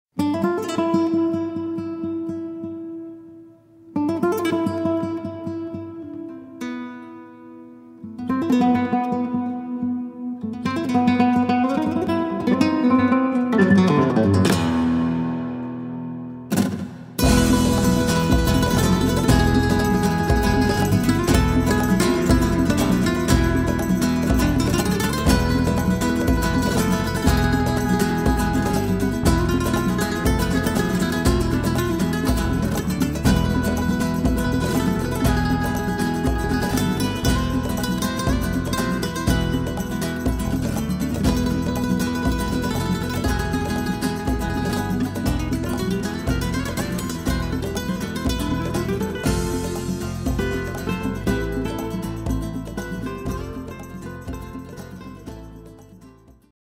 Flamenco Show Live in Bucks County, PA
Suspiro Flamenco Live on October 23 at 3 PM
at the Zlock Performing Arts Center - Bucks County Community College in Newtown, PA.